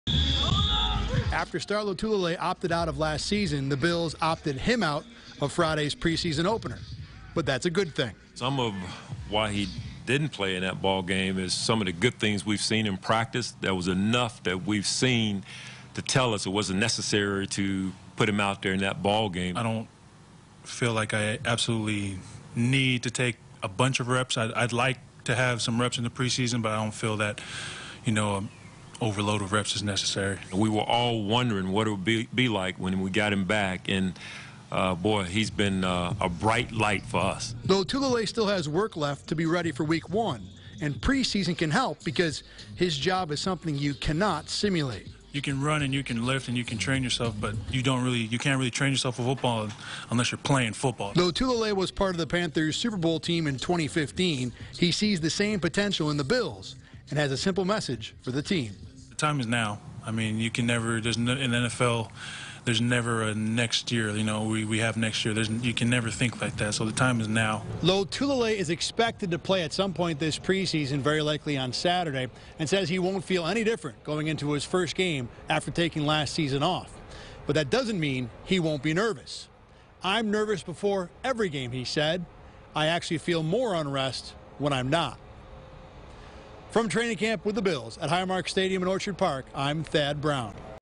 Star Lotulelei shines after a season off
Bills DT Star Lotulelei shines after a season off